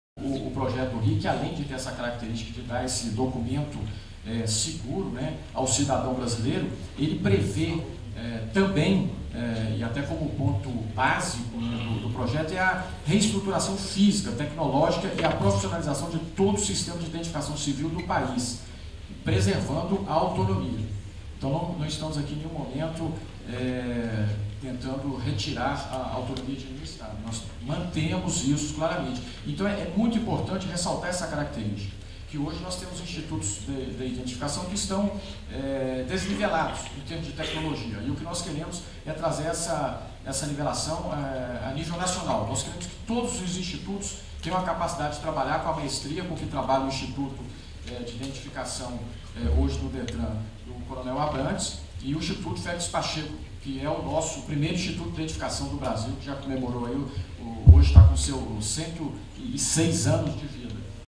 RIC: Diretor do INI garante manutenção do legado
O Diretor do Instituto Nacional de Identificação (INI), Marcos Elias,após palestra do deputado Hugo Leal, tratou de tranquilizar o parlamentar quanto ao modelo de implementação do RIC nos Estados pelo governo federal. Segundo ele, a principal função da proposta será integrar os sistemas e não conflitar com as iniciativas já instaladas e em operação.